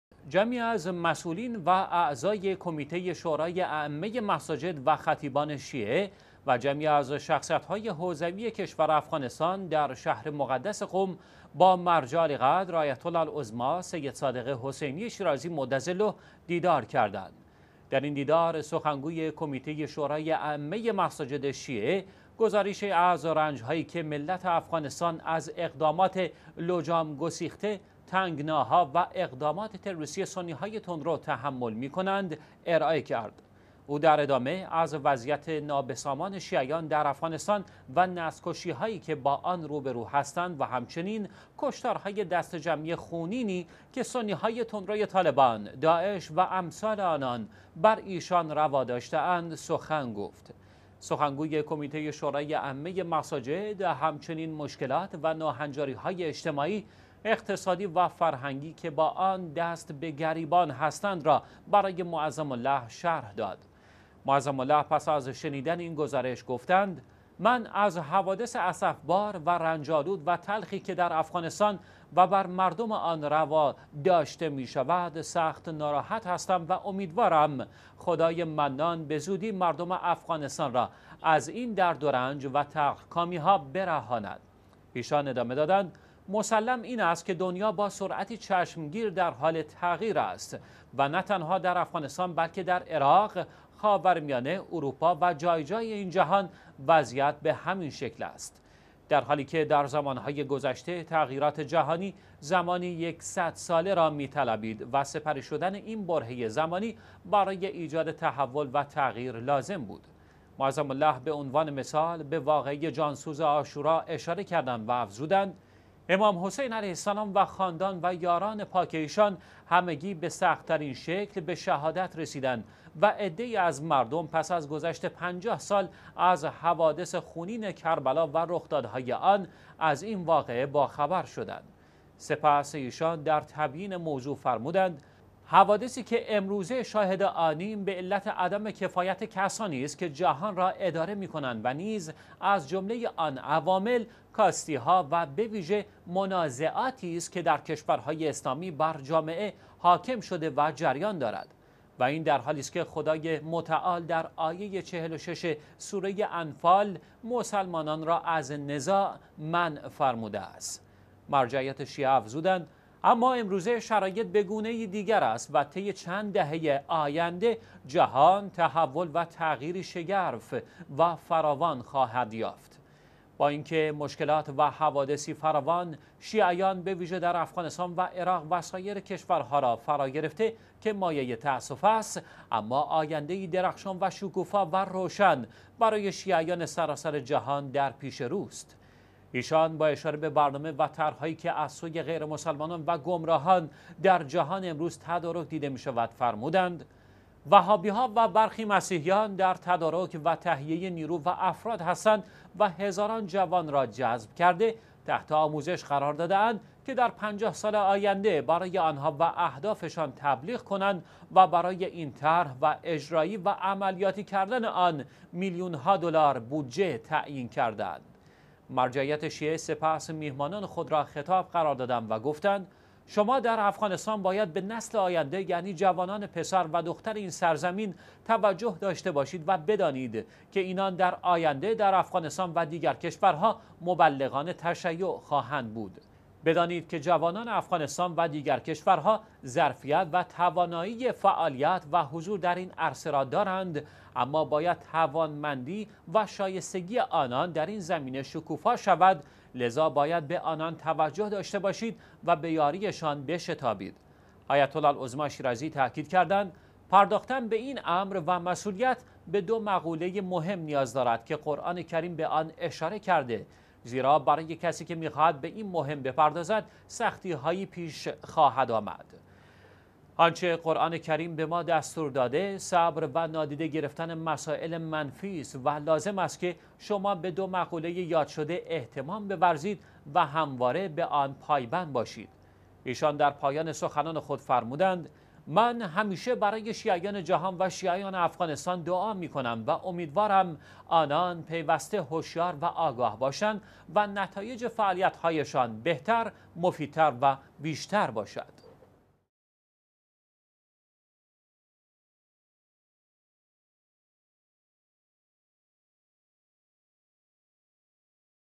دیدار خطیبان و ائمه جماعت مساجد افغانستان، با مرجعیت شیعه آیت الله العظمی شیرازی
جمعی از مسئولان و اعضای کمیته شورای ائمه مساجد و خطیبان شیعه و جمعی از شخصیت های حوزوی کشور افغانستان در شهر مقدس قم با مرجع عالیقدر آیت_الله_العظمی_شیرازی مدظله دیدار کردند.

در این دیدار، سخنگوی کمیته شورای ائمه مساجد شیعه گزارشی از رنج و محنتی که ملت افغانستان از اقدامات لجام گسیخته، تنگناها و اقدامات تروریستی سنی های تندرو با آن رو برو هستند، ارائه نمود.

وی در ادامه از وضعیت نابسامان شیعیان در افغانستان و نسلکشی هایی که با آن روبرو هستند و همچنین کشتارهای دسته جمعی خونینی که سنی های تندروی طالبان، داعش و امثال آنان بر ایشان روا داشته و مشکلات و ناهنجاری های اجتماعی، اقتصادی و فرهنگی که با آن دست به گریبان هستند برای معظم له شرح داد.

به گزارش پایگاه اطلاع رسانی آیت الله العظمی شیرازی، معظم له پس از شنیدن این گزارش گفتند: من از حوادث اسفبار و رنج آلود و تلخی که در افغانستان و بر مردم آن روا داشته می شود سخت ناراحت هستم و امیدوارم خدای منان به زودی زود مردم افغانستان را از این درد و رنج و تلخ کامی ها برهاند.

ایشان ادامه دادند: مسلّم این است که دنیا با سرعتی چشمگیر در حال تغییر بوده و نه تنها در افغانستان، بلکه در عراق، خاورمیانه، اروپا و جای جای این جهان وضعیت به همین شکل است، در حالی که در زمان های گذشته تغییرات جهانی زمانی یکصد ساله را می طلبید و سپری شدن این برهه زمانی برای ایجاد تحول و تغییر لازم بود.

معظم له به عنوان مثال به واقعه جانسوز عاشورا اشاره نموده و افزودند: امام_حسین علیه السلام و خاندان و یاران پاک ایشان همگی به بدترین وجه به شهادت رسیدند و عده ای از مردم پس از گذشت پنجاه سال از حوادث خونین کربلا و رخدادهای آن با خبر شدند.

متن کامل خبر در کانال شیعه ویوز (لینک در بیو)